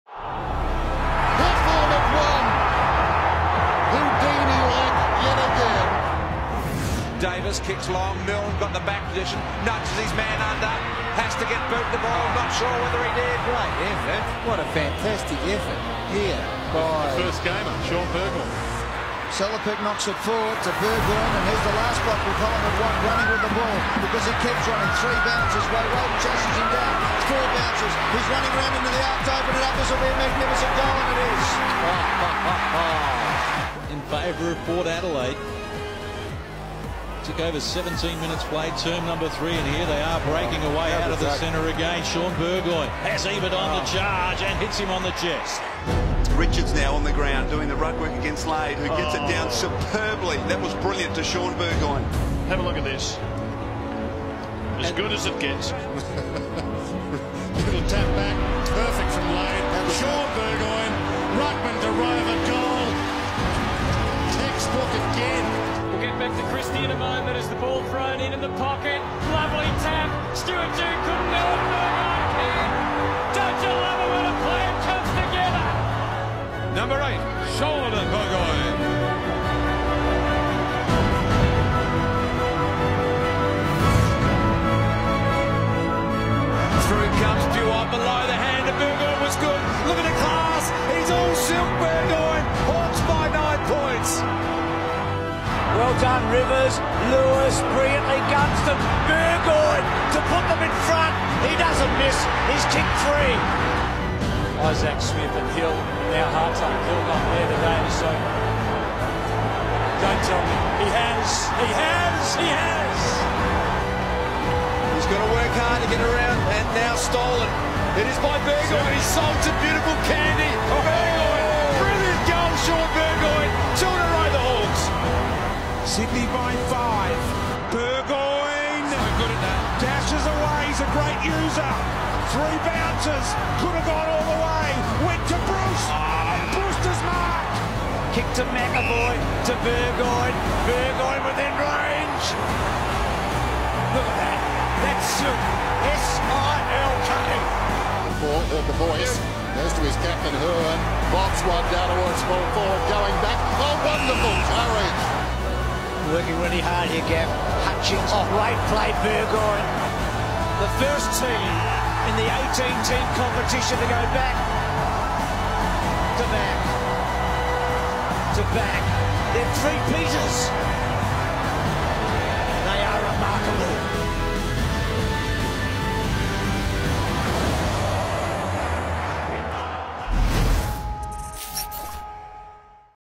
350 games of pure Silk from Burgoyne | Round 13, 2018 | AFL
Across a stellar career with Port Adelaide and Hawthorn, this man has produced quite the showreel, including four premierships.